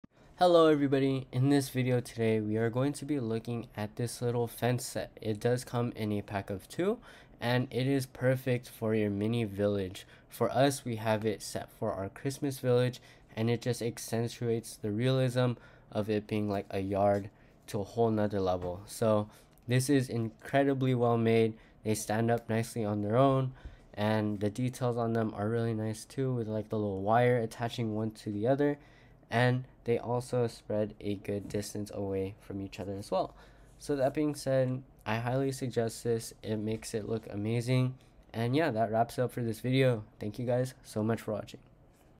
Miniature Fence Decorative Picket Fence Showcase
Showcasing the Miniature Fence Decorative Picket Fence from our Christmas village
Link to product